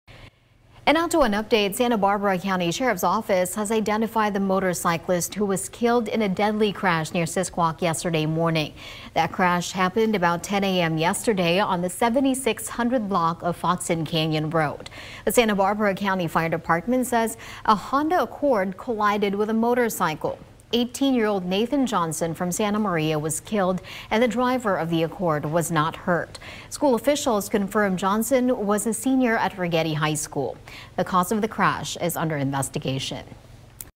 Motorcyclist killed in crash near Sisquoc was high school senior
The Santa Barbara County Sheriff's Office has identified the motorcyclist who was killed in a crash near Sisquoc on Sunday as Nathan Johnson, 18, of Santa Maria.